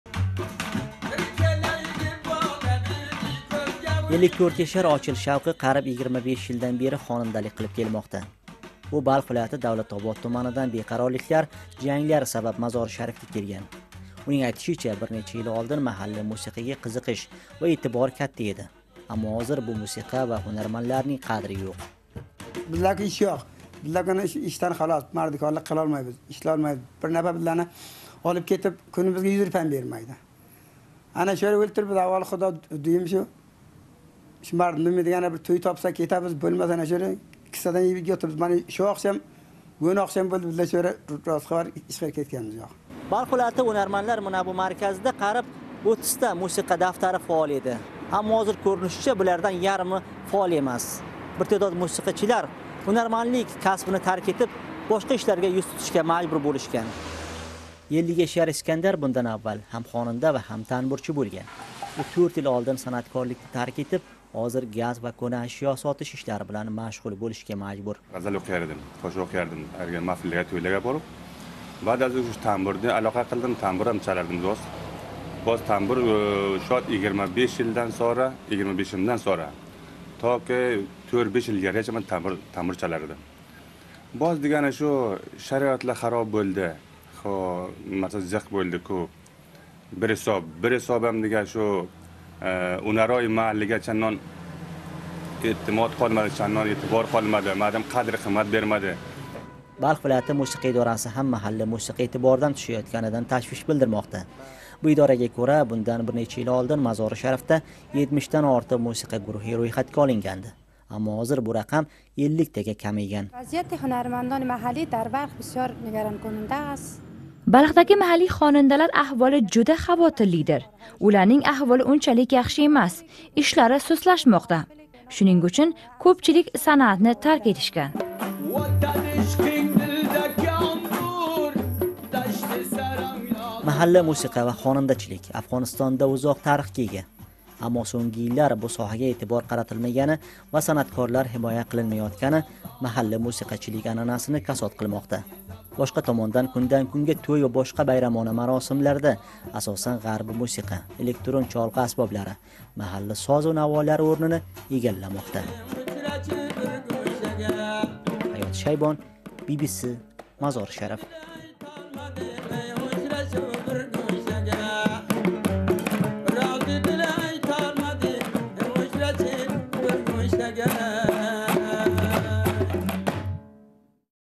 Қайси ўзбек хонандаси эскифурушлик қилиб кун кўрмоқда? BBC Uzbek
Ўзбекистонда хонандалик даромадли соҳа.

Тўй ва турли зиёфатларда бир нечта ашула айтиб, юзлаб доллар ишлаш мумкин.

Аммо ҳамма жойда ҳам ўзбек хонандаларининг аҳволи яхши эмас.

Қатор истеъдодли хонанда ва мусиқачилар амаллаб кун кўришмоқда.

Би-би-си Ўзбек Хизмати  тайёрлаган ушбу видео лавҳа бозори касод бўлган ва бугун ишсиз қолган ўзбек хонандалари ва мусиқачилари ҳақида.

Хусусан, экси буюмларни сотиб оила боқаётган  хонанда ва мусиқачи тақдири ҳақида ҳикоя қиламиз.